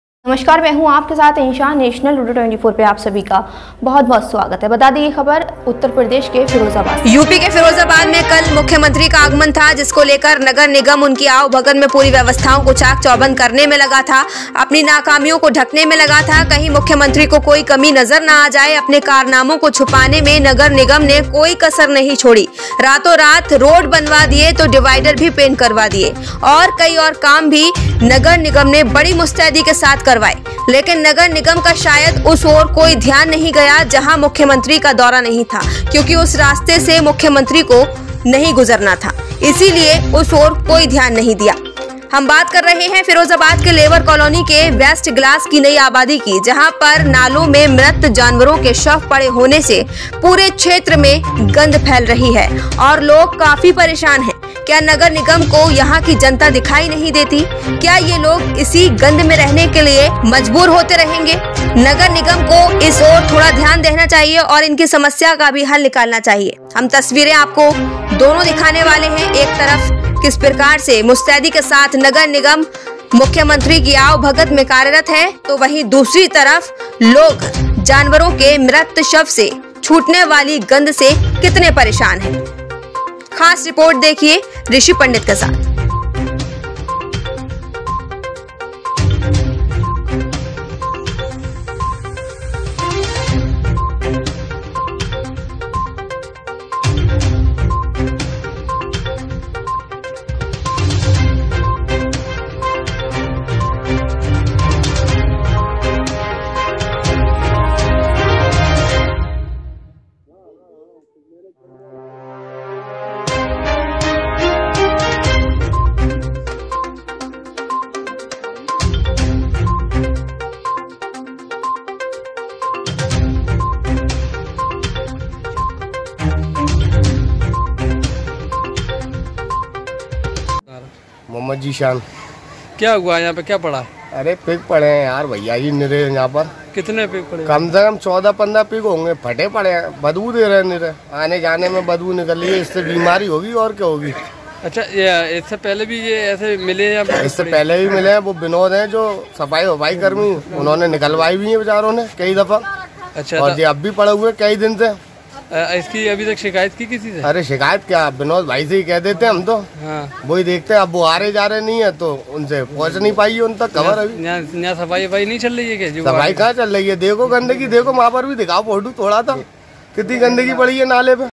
nagarnigam लेबर कॉलोनी के लोग सड़ते हुए जानवरो की गंदी बदबू से परेशान रहे, नगर निगम छलावा करता रहा
एक तरफ सूबे के मुखिया से डरा-डरा दिखाई दिया नगर निगम, तो लेबर कॉलोनी के लोगो के लिए लापरवाह दिया दिखाई।